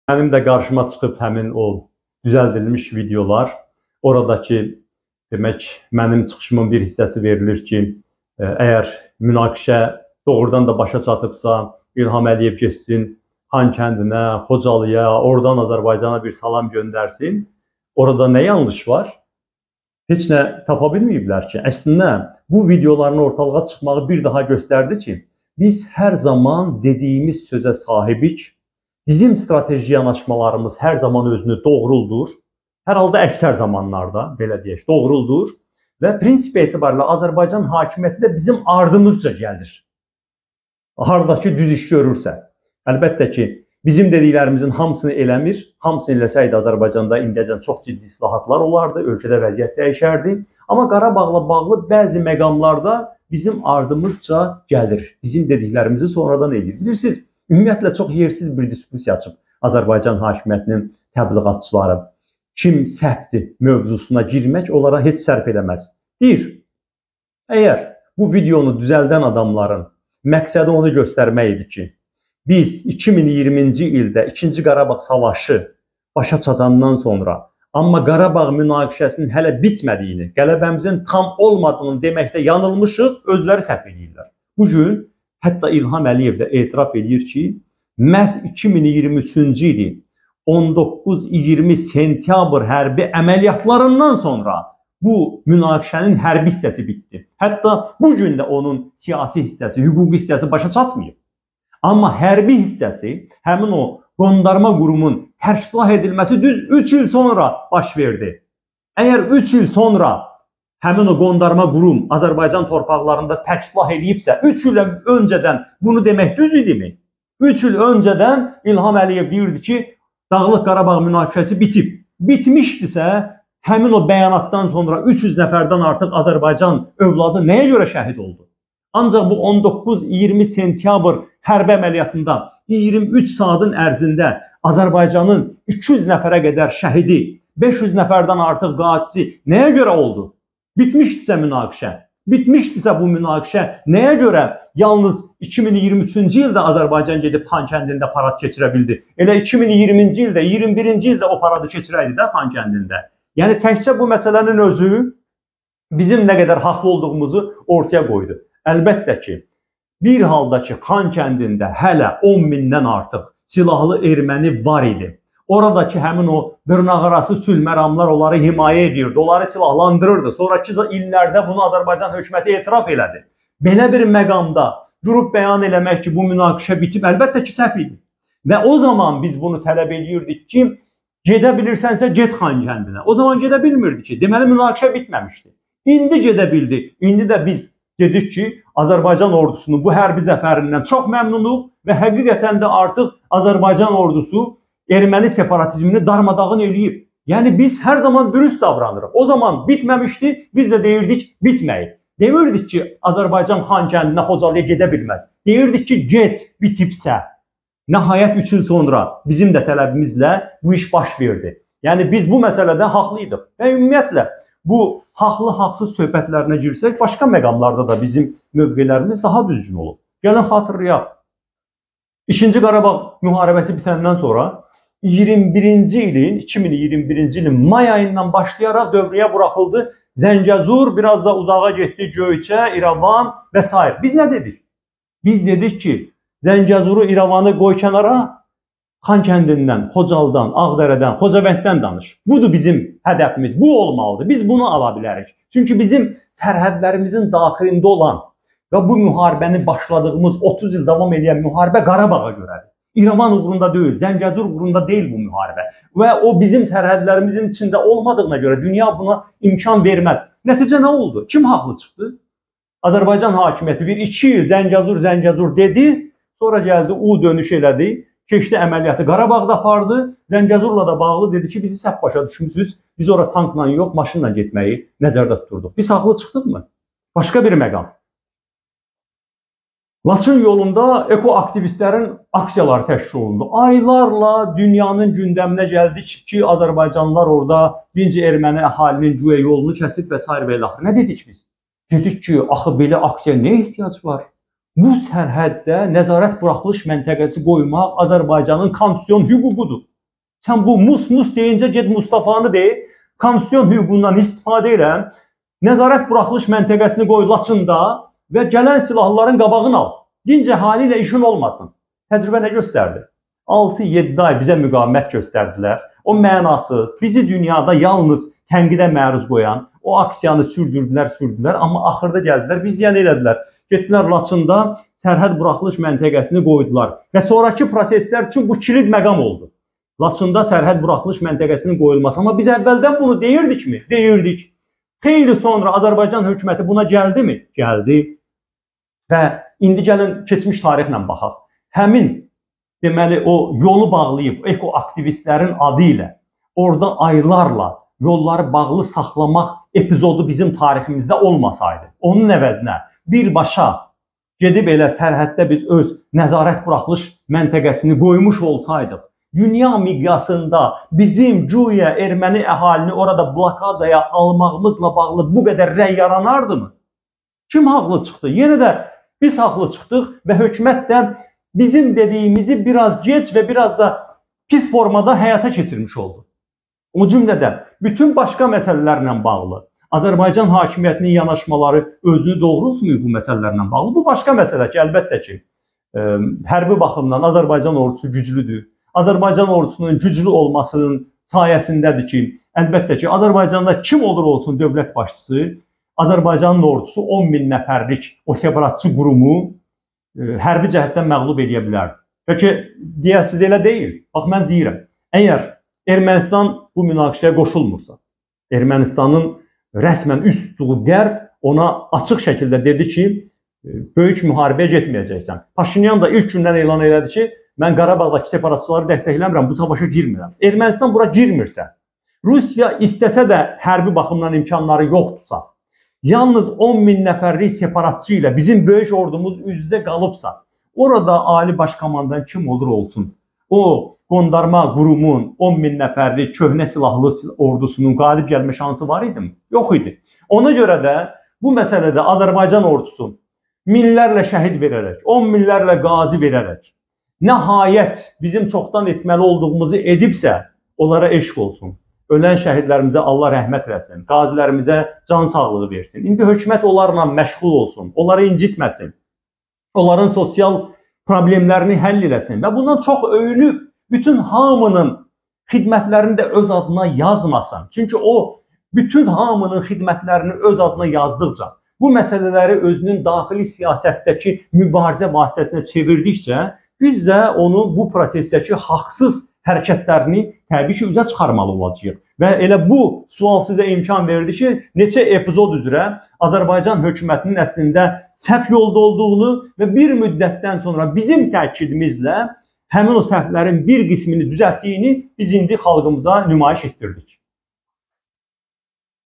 Bu videolar bizim doğru dediyimizin etirafıdır Əli Kərimli yayılan videolara belə münasibət bildirdi
Xankəndinə Azərbaycan bayrağı sancılandan sonra rejim xislətinə uyğun olaraq bu milli qələbəni də öz siyasi rəqiblərinə - şəxsən sizə  qarşı istifadə etməyə çalışdı. Əksər sosial şəbəkələrdə hökumətin nəzarətində olan səhifələr pullu reklamla belə bir video paylaşırdılar. Videonun əvvəlində 2-ci Qarabağ savaşından sonrakı dövrdə sizin etdiyiniz çıxışlardan belə bir hissə gedir ki, Qarabağ münaqişəsi bitibsə, İlham Əliyev getsin, Xankəndinə, Xocalıya... sonra da videonun 2 -ci hissəsi gəlir və orada İlham Əliyev Xanləndində bayraq qaldırır və deyir ki, nəyi nə vaxt necə etməyi mən bilirəm. Hökumət təbliğatçıları bu yolla sizi səhv çıxarmağa, İlham Əliyevin isə hər zaman düz olduğunu sübut etməyə çalışıblar. Bu videolar haqqında nə deyə bilərsiz?